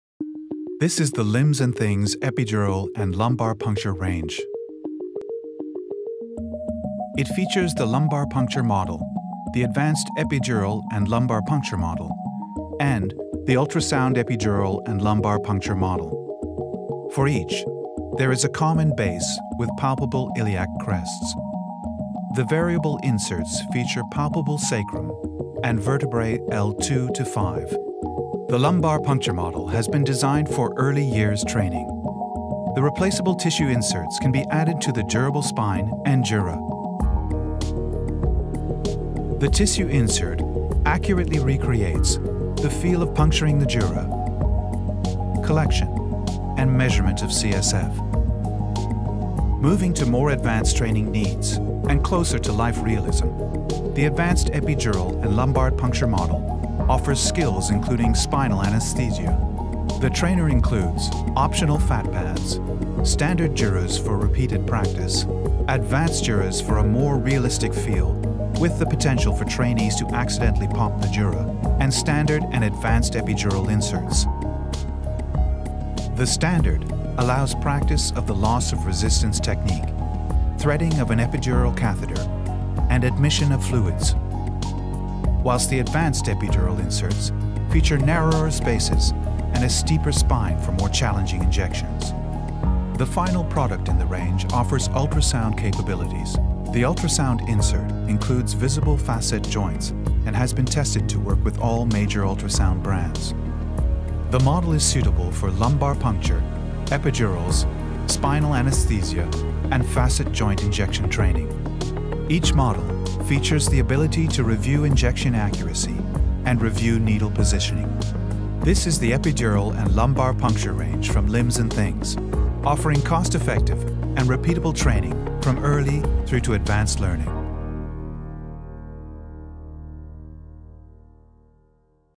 腰椎穿刺 和硬膜外系列
本系列是针对使用触诊和超声引导术进行
腰椎穿刺和硬膜外麻醉的核心技能训练而
设计的。 本系列提供逼真的、可重复的和
极具成本效益的训练。

三种模型的底座通用，但可提供不同的复
杂程度，因此从早期入门到深入学习的学
员均可使用。

与临床医师共同设计，这些模型可提供逼
真的触感和反应，同时提供了一个安全的
学习和技能保持环境。